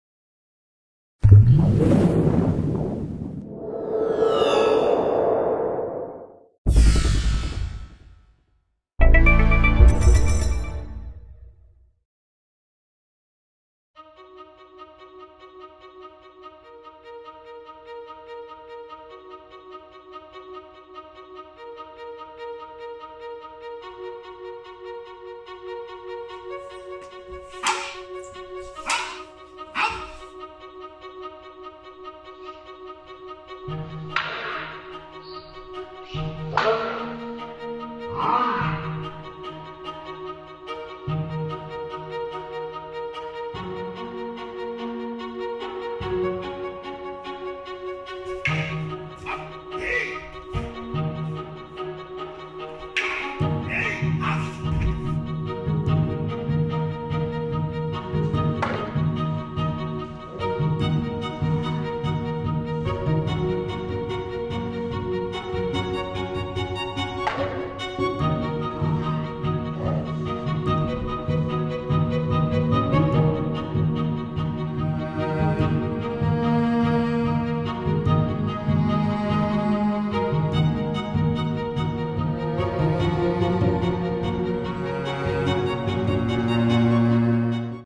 (2*) Ken tai jo ni henka 5 - Marco Rubatto
Variazione del 2º Ken tai jo,  forma di armonizzazione a con proiezione (taijutsu no riai), basata sul 7º kumijo

2º Ken tai jo variation, stick harmonization form (taijutsu no riai), based on 7th kumijo